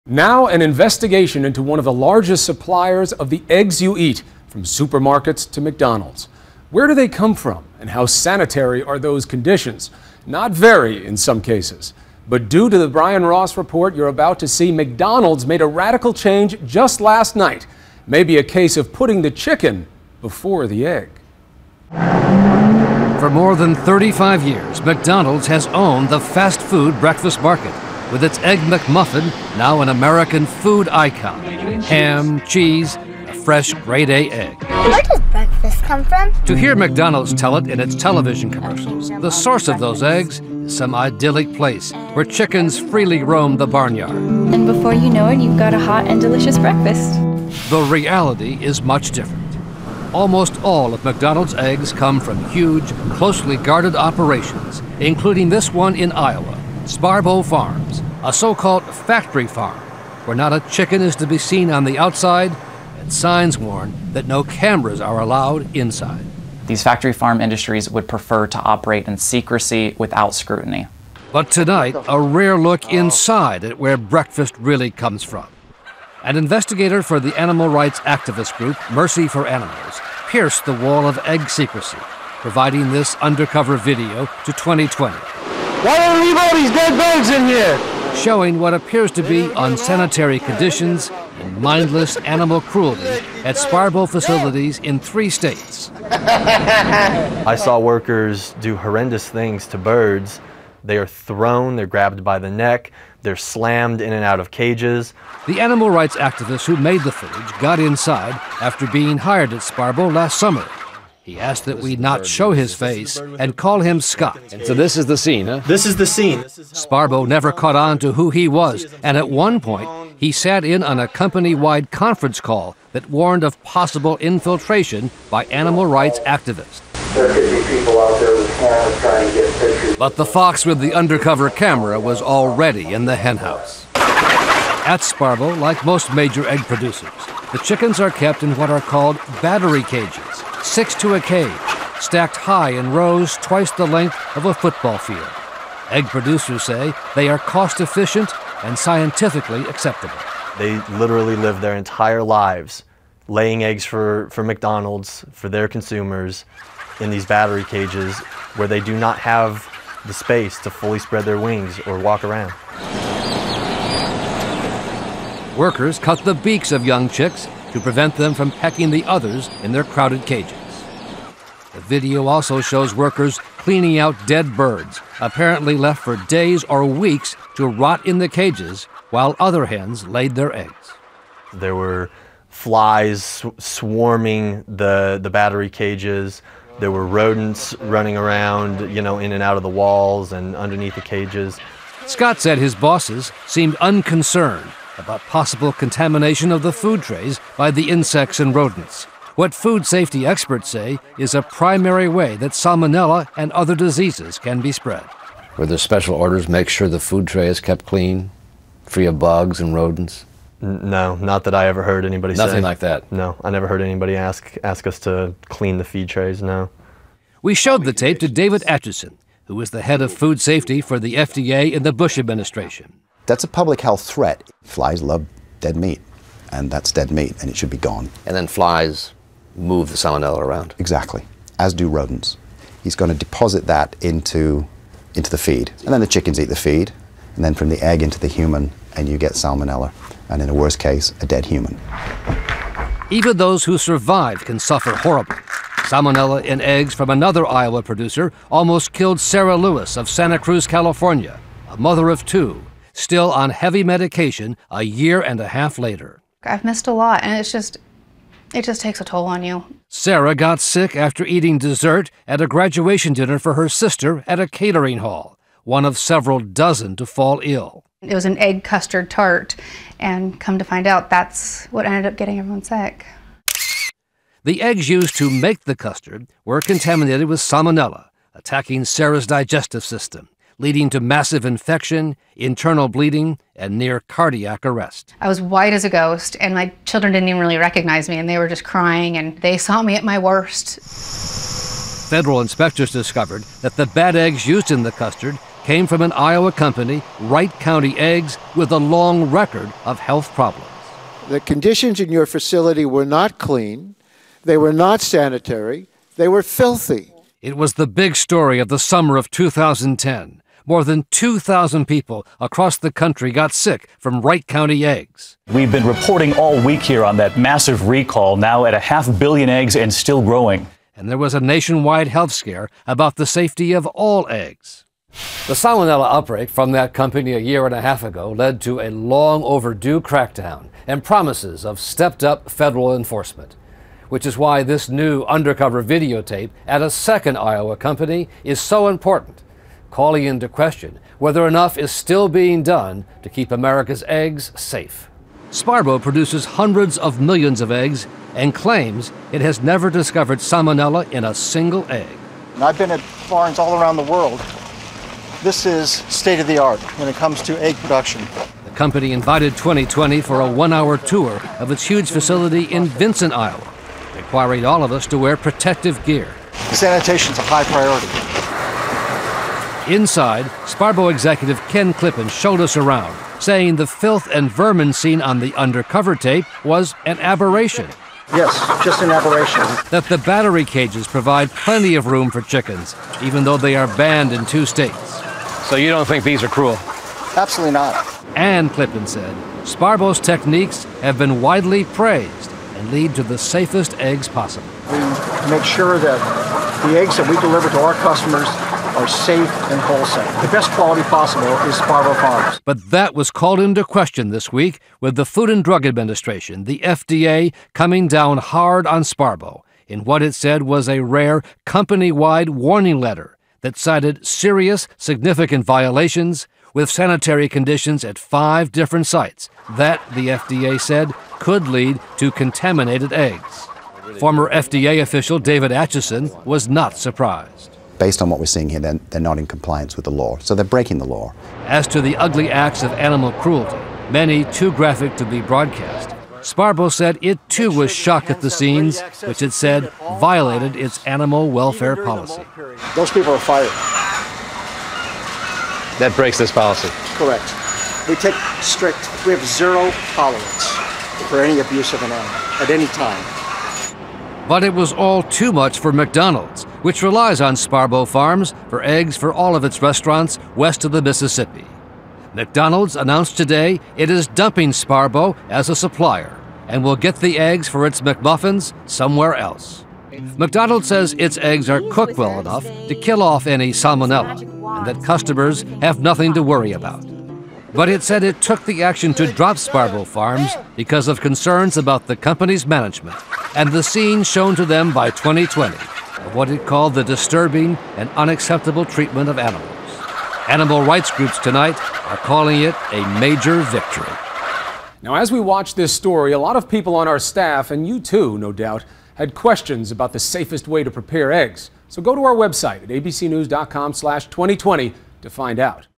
Investigation: Inside Egg 'Factory Farm'
Animal rights group video appears to show unsanitary conditions, animal cruelty.